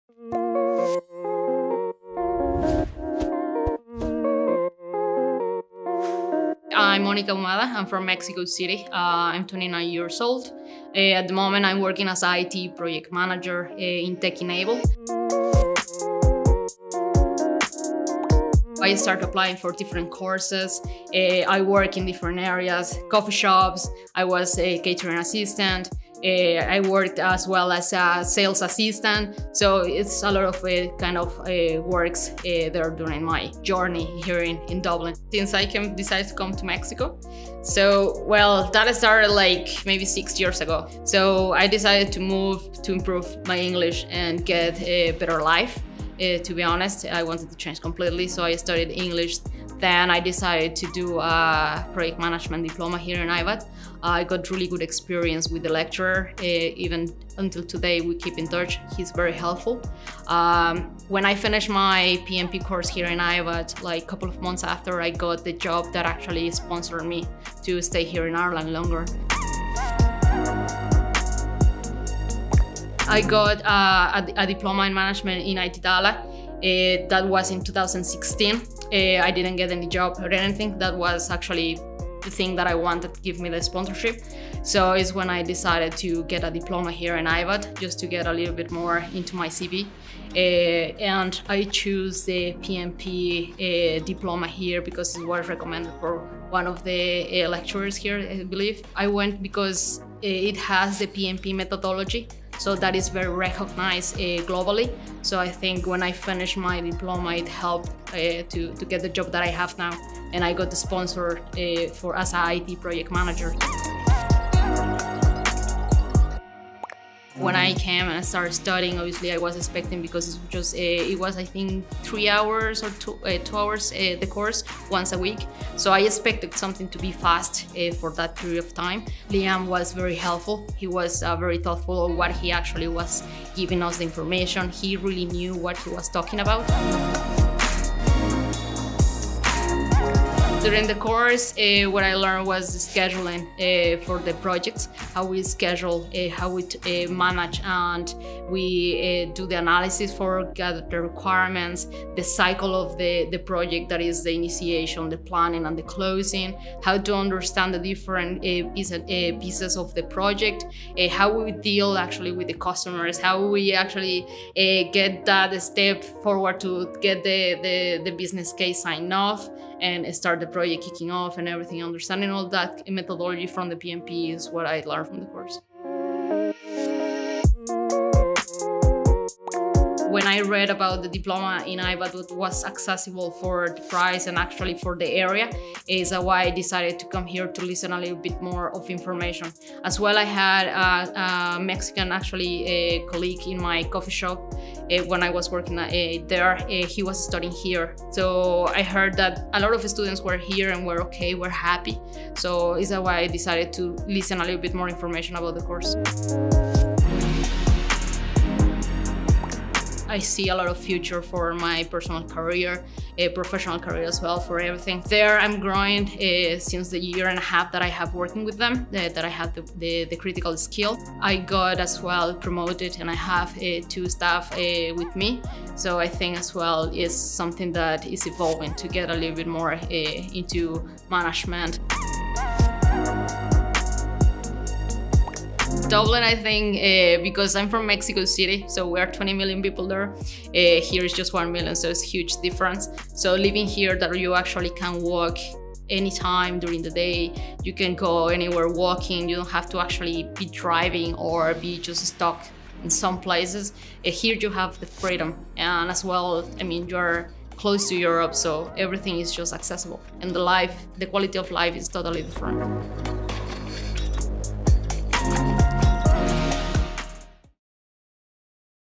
Meet Monica Ahumada, English Language & Diploma in Project Management Graduate | IBAT College Dublin
Meet graduate Monica Ahumada. In this video, she shares her experiences of studying for a Project Management diploma and talks about what life is like as a student at IBAT College Dublin. She explains her educational journey whilst learning in Dublin, saying that she first chose to study English language in order to start a better life. She talks about pursuing the Project Management diploma course at IBAT which has helped her shape her career. Monica outlines what she learnt during the course such as managing and scheduling projects, as well as analysing and understanding the project management cycle. She also talks about why she joined IBAT and how she found Dublin different from her home city in Mexico.